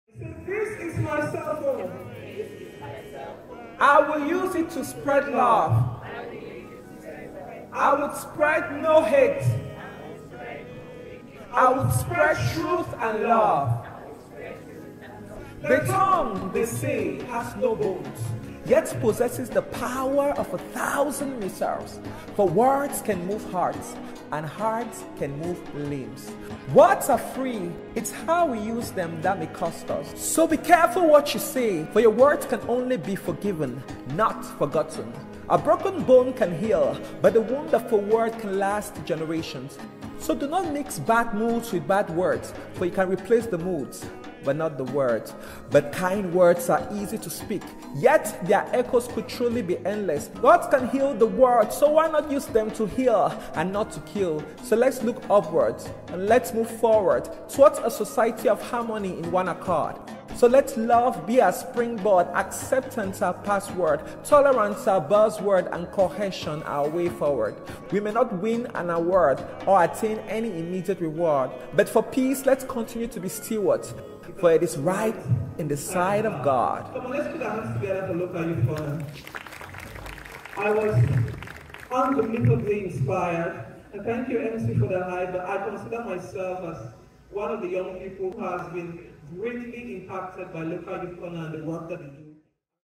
W-O-R-D-S Live Spoken Word Performance by Mac Alunge
MacAlunge performs SpokenWord Poetry at Palais de Sports -Yaounde at Local Youth Corner Cameroon's 2023 Na We We Sports Jamboree Closing Gala Event.

A great honour it was performing with some of Cameroons biggest Artists Senior Pastor Comedian  and Wax Dey in audience of Haitem Zaiter of X-Maleya, Sir Marcus Davies of High Commission of Canada to Cameroon, De Oracle Comedian, Sir Ngala Desmond of Defyhatenow_WCA and other dignitaries. Thank you to the Executive Director of LoYoC Achaleke Christian...